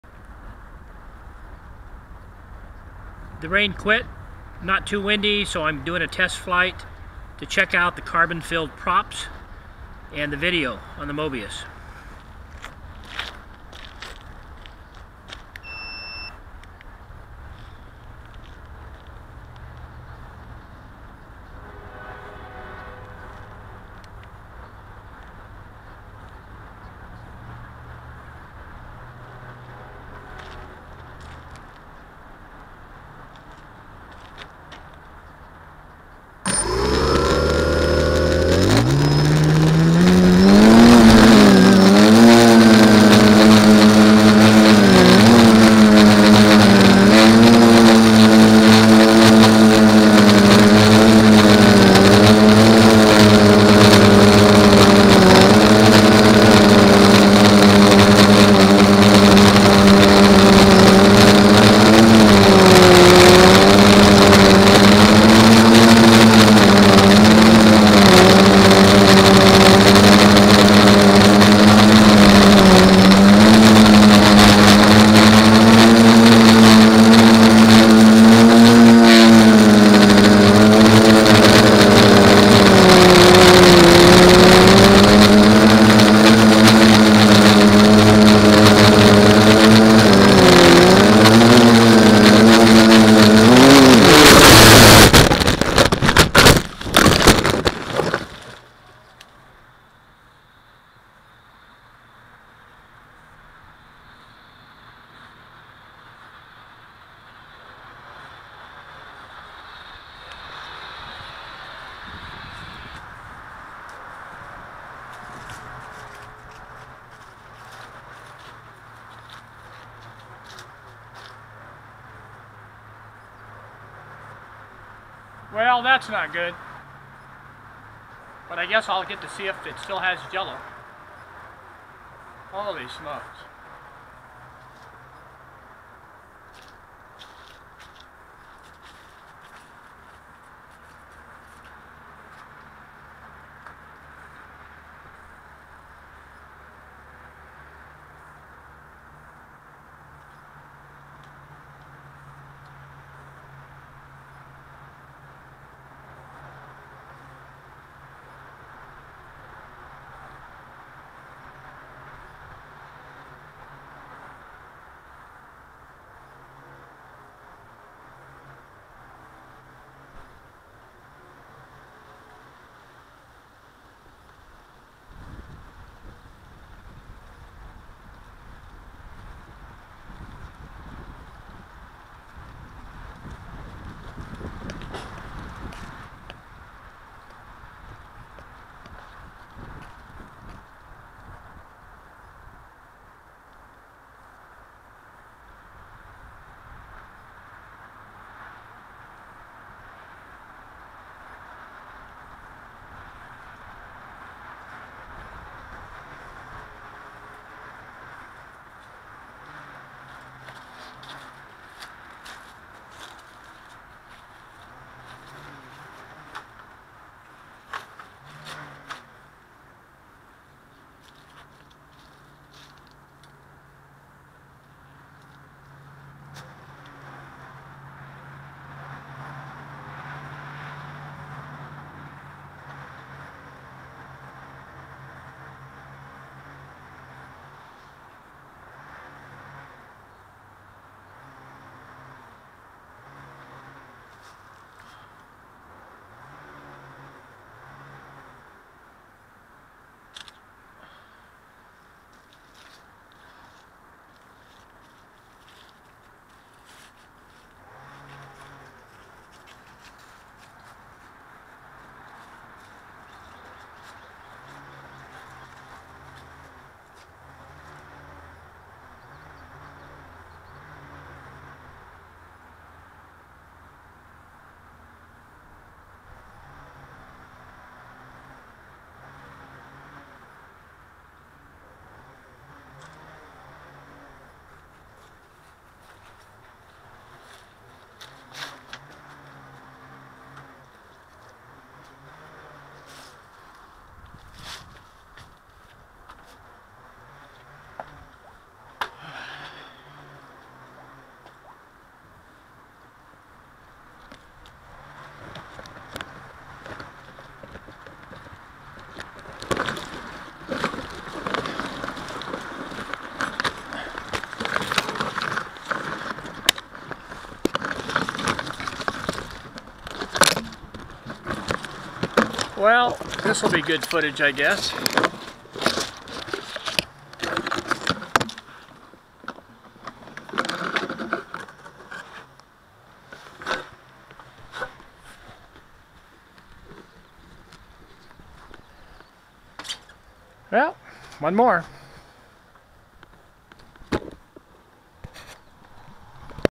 FPV Carbon Prop Video Test Ends in a Tree
Test of Carbon Filled Props to Prevent Video "Jello"